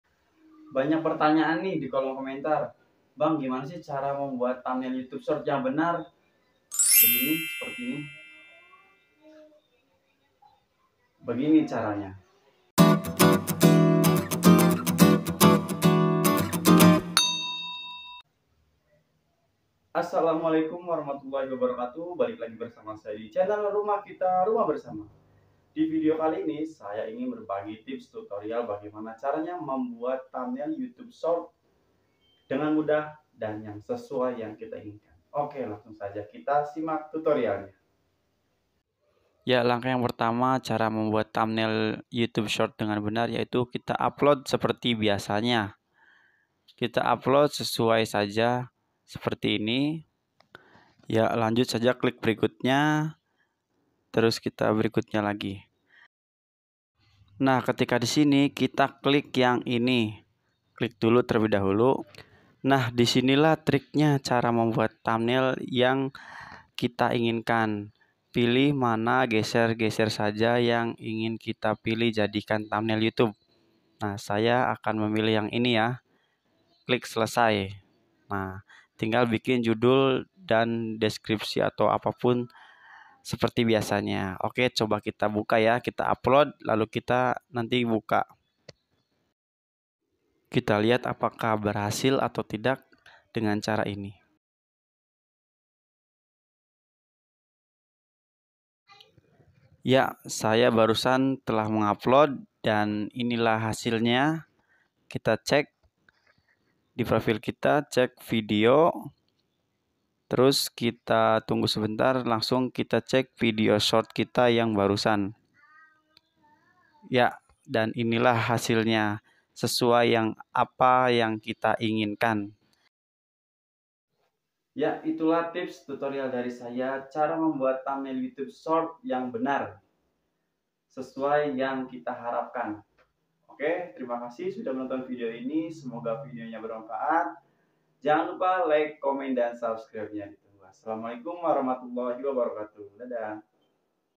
Cara pasang / mengganti Thumbnail Untuk Video YouTube shorts Edit lewat Hp di tahun 2023
Berikut Cara ganti pasang Thumbnail YouTube shorts  dengan mudah sesuai yang kita inginkan cara nya sangat mudah kita bisa lakukan sendiri lewat hp tanpa aplikasi.

Mengatasi masalah pada YouTube shorts tidak bisa ganti thumbnail dan inilah solusinya terbaru di tahun 2023